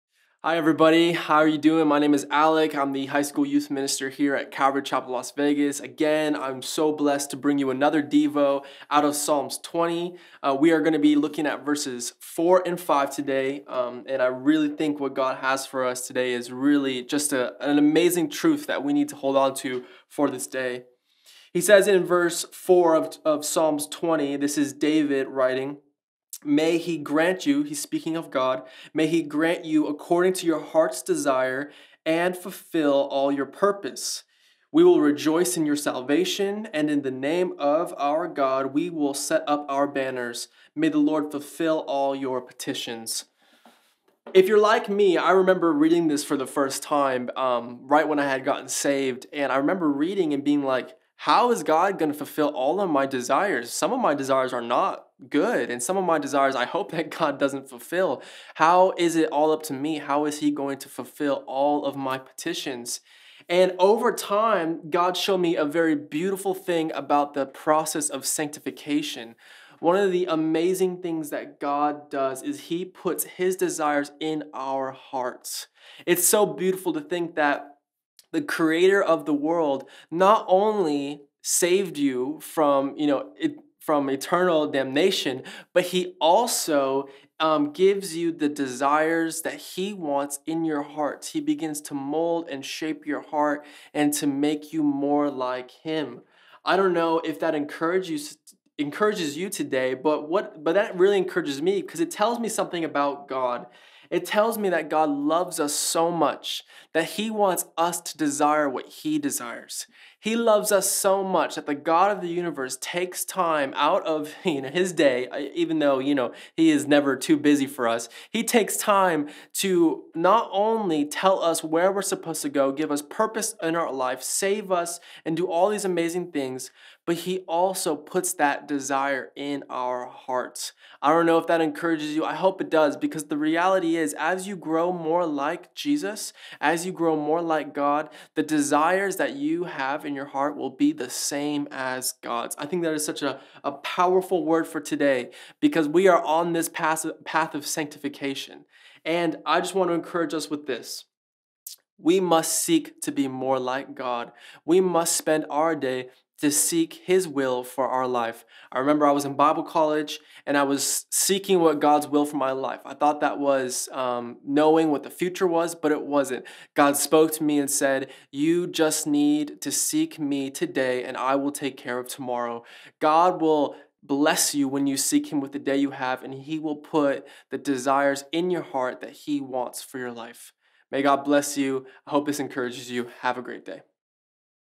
Calvary Chapel Las Vegas | Daily Devotion | Alec Neider | Psalm 20:4-5
May He grant you according to your heart’s desire, And fulfill all your purpose. We will rejoice in your salvation, And in the name of our God we will set up our banners! May the LORD fulfill all your petitions.
Psalms‬ ‭20:4-5‬